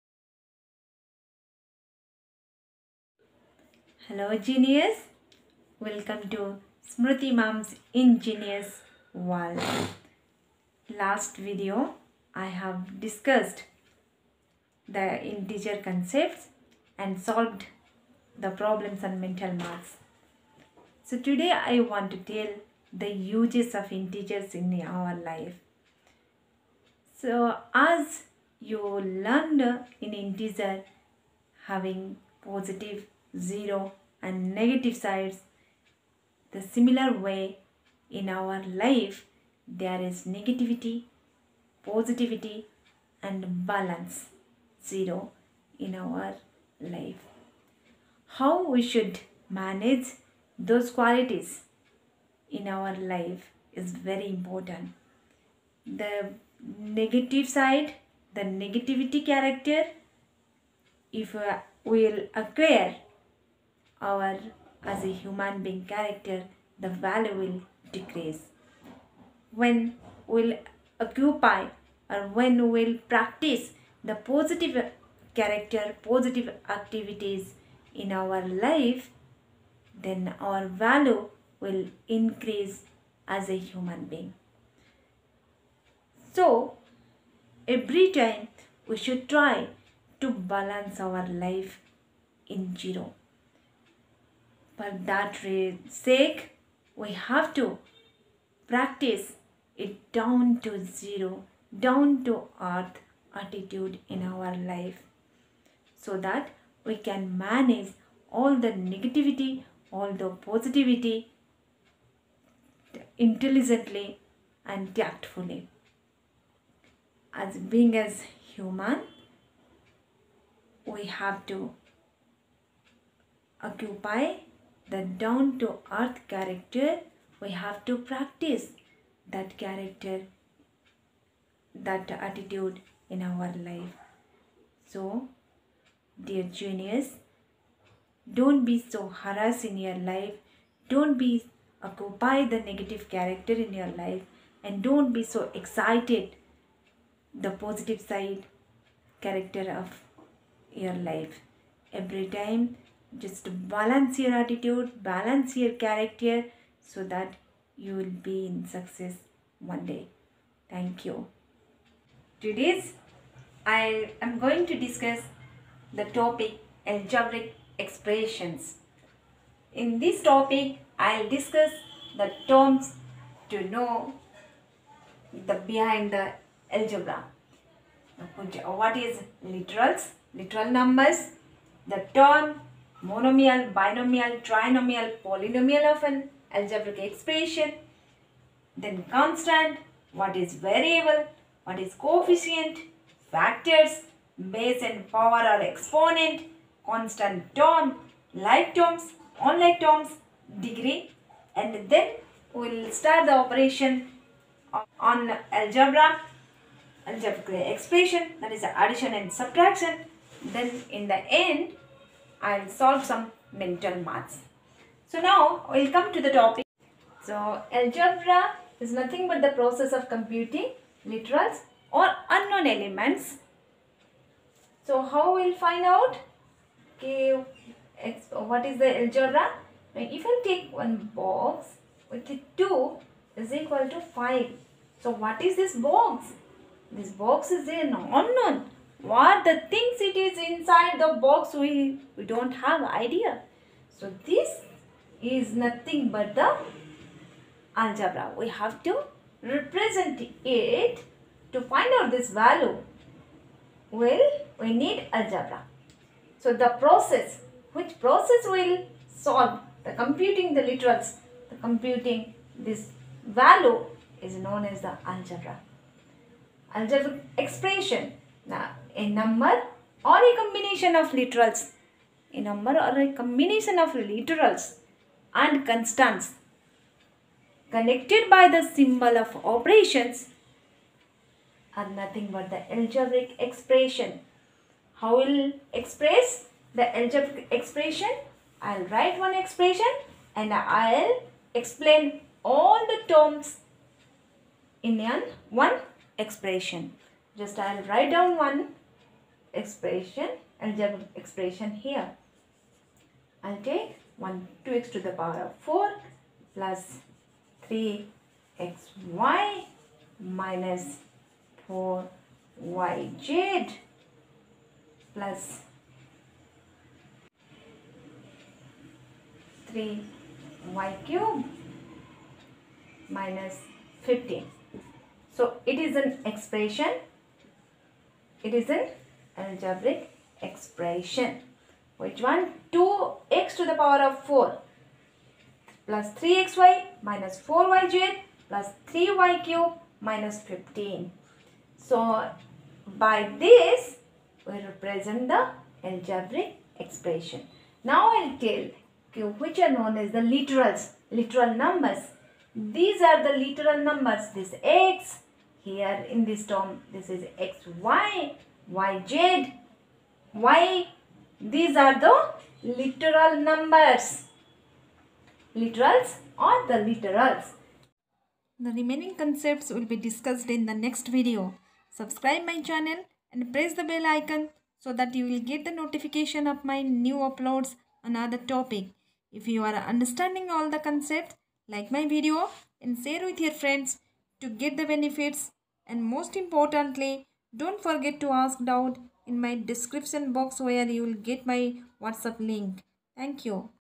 PART -1: Algebraic expression//Terms to know//Addition and Subtraction//mental maths solution
This is part -1: of Algebraic expression. It describes
   - definition of algebra
   - definition of algebraic expression
   - literals